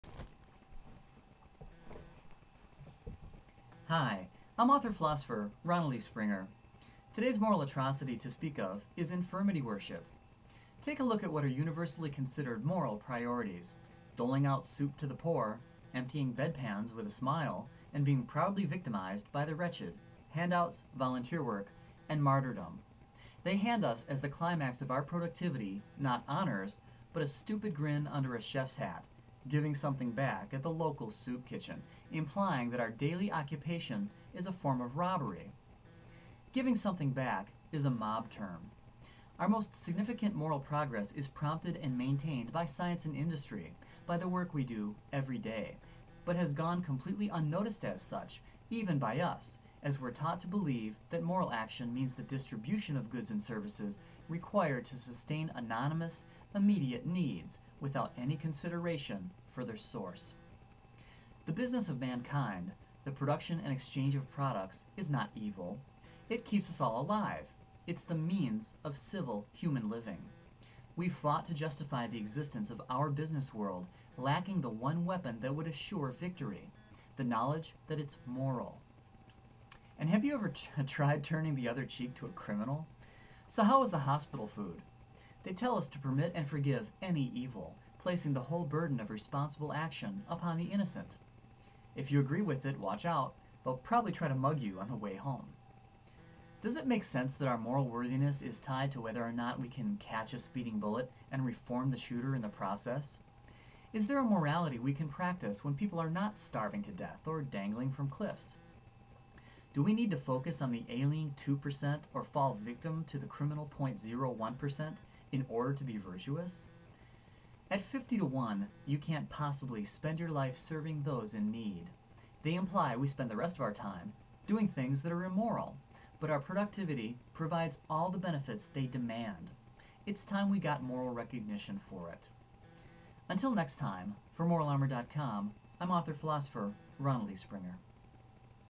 Armour of God or Nature's Moral Armor? Infirmity Worship Vid #8
Armour of God or Nature's Moral Armor? Infirmity Worship

Moral Armor on Amazon

The Care and Feeding of Freedom on Amazon

to learn more about the philosophy of Moral Armor.

What is the first moral consideration for Man; the fittest or the weakest? Who has moral superiority?

MORAL ARMOR’S Infirmity Worship.

The last moral atrocity I'll speak of is infirmity worship. Take a look at what are universally considered moral priorities: doling out soup to the poor, emptying bed-pans with a smile and being proudly victimized by the wretched: hand-outs, volunteer work and martyrdom. They hand us as the climax of our productivity not honors, but a stupid grin under a chef’s hat, “giving something back” at the local soup kitchen, implying our daily occupation is a form of robbery. "Giving something back" is a mob term. Our most significant moral progress is prompted and maintained by science and industry--by the work we do everyday--and has gone completely unnoticed as such, even by us, as we’re taught to believe that moral action means the distribution of goods and services required to sustain anonymous, immediate needs, without any consideration for their source. The business of mankind—the production and exchange of products—is not evil; it keeps us all alive. It’s the means of civil human living. We've fought to justify the existence of our business world, lacking the one weapon that would assure victory; the knowledge that it’s moral.

And have you ever tried "turning the other cheek" to a criminal? So how was the hospital food? They tell us to permit and forgive any evil, placing the whole burden of responsible action upon the innocent. If you agree with it, watch out! They'll probably try to mug you on the way home. Does it make sense that our moral worthiness is tied to whether or not we can catch a speeding bullet and reform the shooter in the process? Is there a morality we can practice when people are not starving to death or dangling from cliffs? Does we need to focus on the ailing two percent and fall victim to the criminal .01% in order to be virtuous? At fifty-to-one, you can’t possibly spend your life serving those in need. They imply we spend the rest of our time doing immoral things, but our productivity provides all the benefits they demand. It’s time we got moral recognition for it.

To learn more about the philosophy of Moral Armor, please advance to the next video in the Armor of God or Nature's Moral Armor? series!